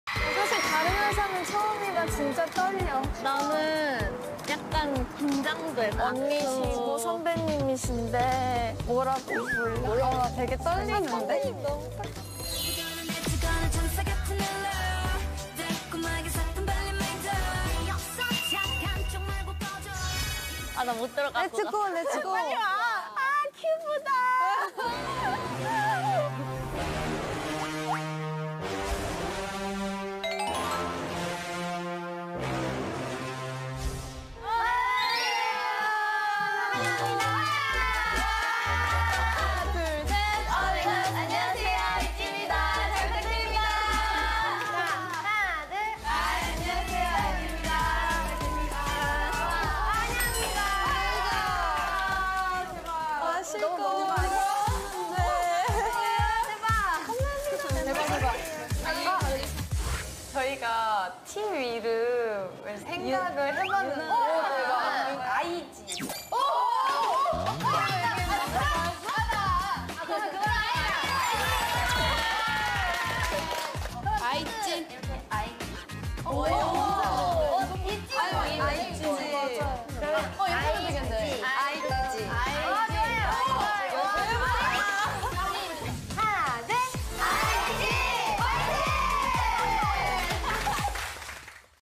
Itzy visting Cube Entertainment to meet Gidle
Itzy Gidle